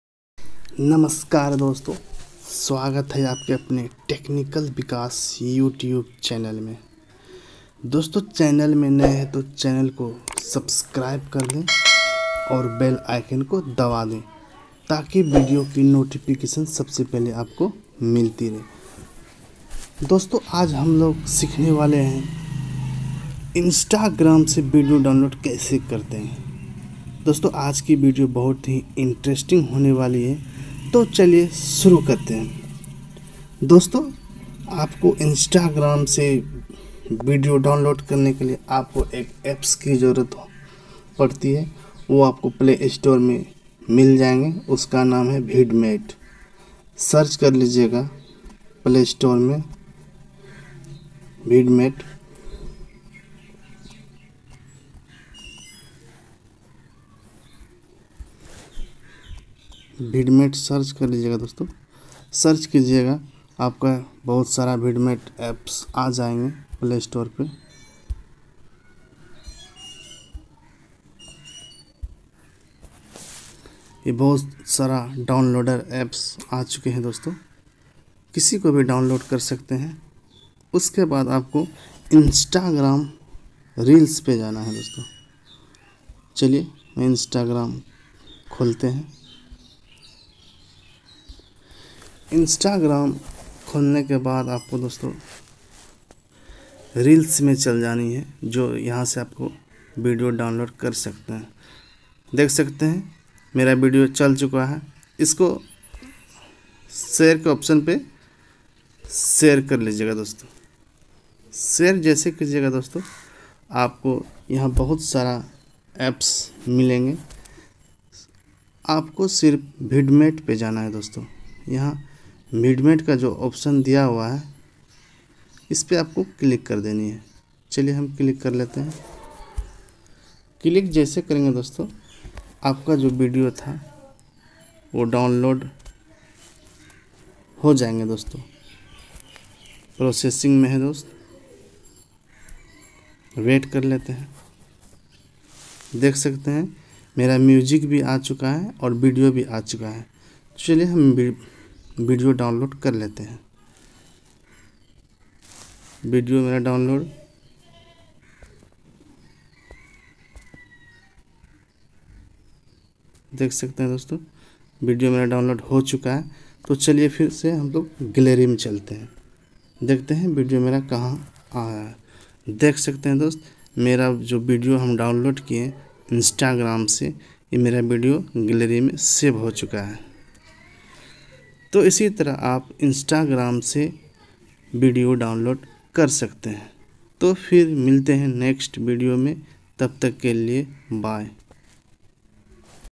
Instagram Se video download kaise karen | how to download Instagram videos
Jagran video gallery Mein Kaise laen
kya aap Instagram ki video download karna chahte hain to yah video aap Dekhte rahiye